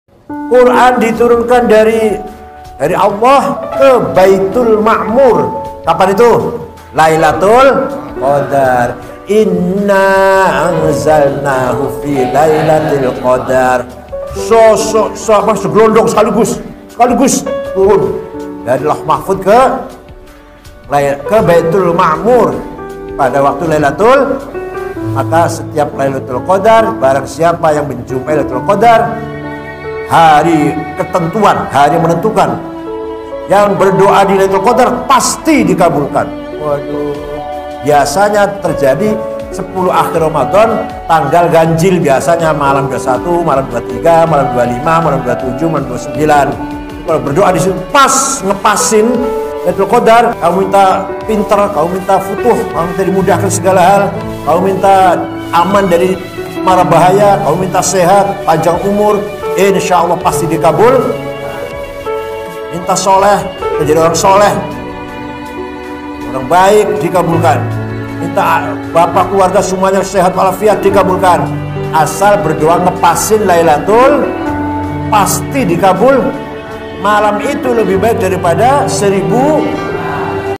Malam Lailatul Qodar
Barang siapa yang bedoa pada malam lailatul qodar pasti dikabulkan. Biasanya 10 malam terakhir romadhon tanggal ganjil.